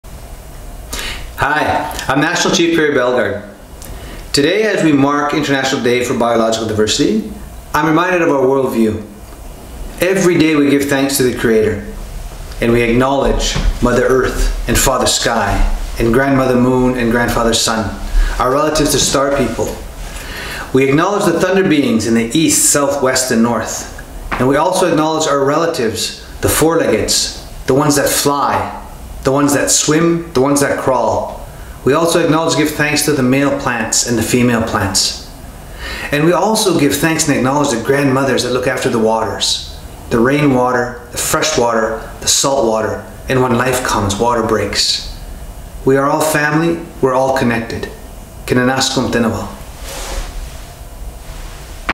Message from Canada’s Assembly of First Nations National Chief on Biodiversity Day 2020
“We are all family; we are all connected” said Mr. Perry Bellegarde, Canada’s Assembly of First Nations National Chief commemorating BiodiversityDay 2020 under the theme of “Our solutions are in nature”. While we are reminded of the close ties between nature and indigenous peoples, we recognise that traditional knowledge of indigenous peoples and local communities provides substantial knowledge for the protection, management, restoration and sustainable use of ecosystem.